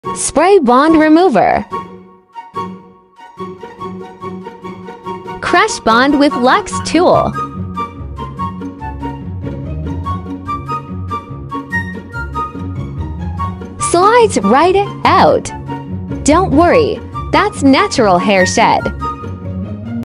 Removing Fusion Bond Hair Extensions
How to easily remove fusion keralink hair extensions.

You know you want that extra big ass color brush